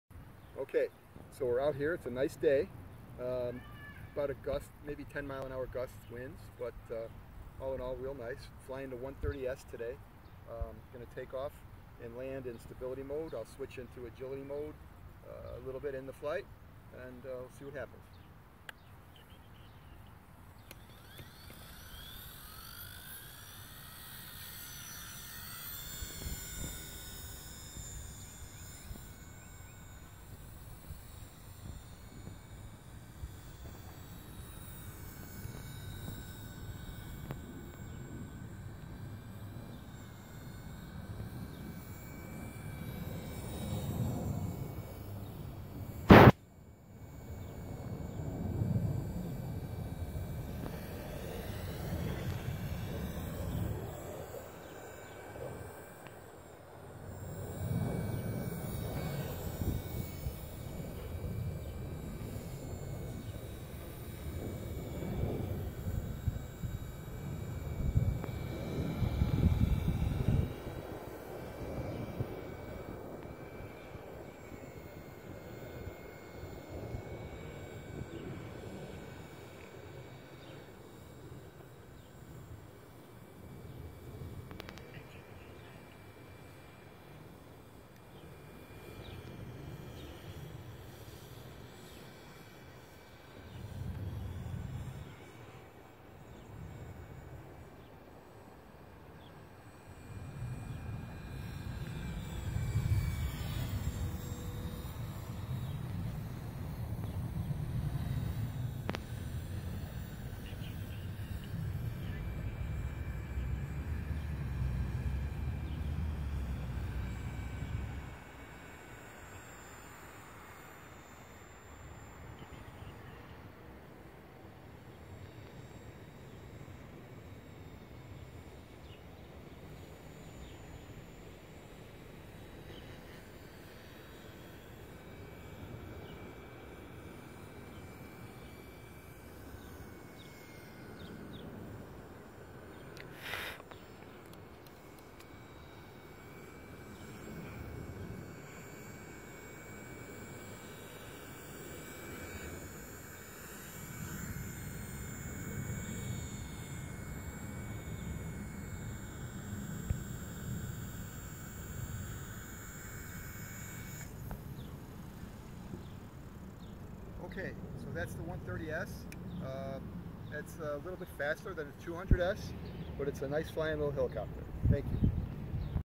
Blade 130S Beginner Flight
Flying the blade 130s helicopter on a nice calm day , and taking off and landing in stability mode.